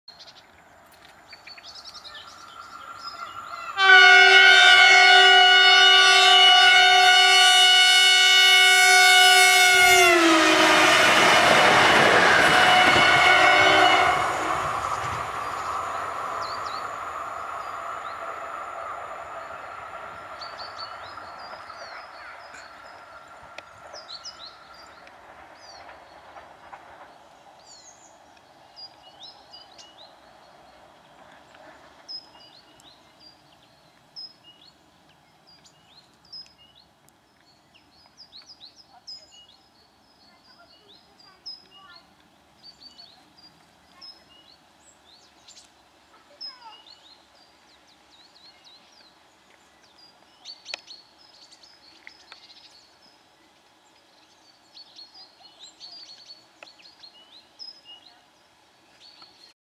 Efektowny przejazd Elfa przez Stacje Kobiór (RP1)
Elf przejeżdża przez Stacje Kobiór.